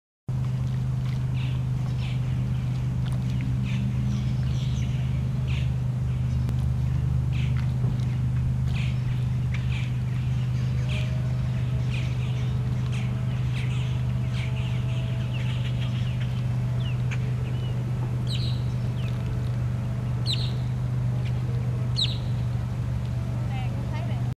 Florida Bird 2
I'm not a Zoologist, but I think this bird is drying his/her wings and feathers.